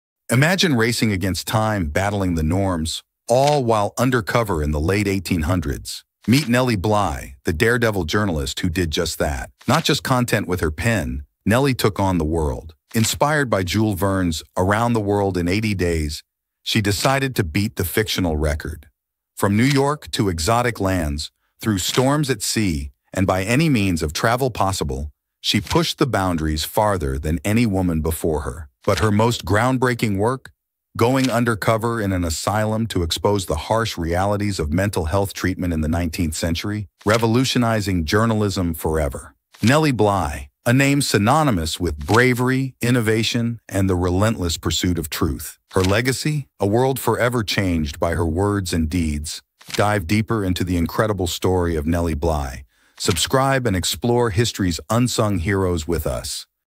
How Nellie Bly Became the World's Most Famous Investigative Reporter!
Did you know that in 1887 Nellie Bly, a fearless journalist went undercover to expose shocking truths? Discover how she faked insanity to infiltrate an asylum, sparking reform and becoming a pioneer in investigative reporting. Don't miss this summary of bravery and the fight for justice!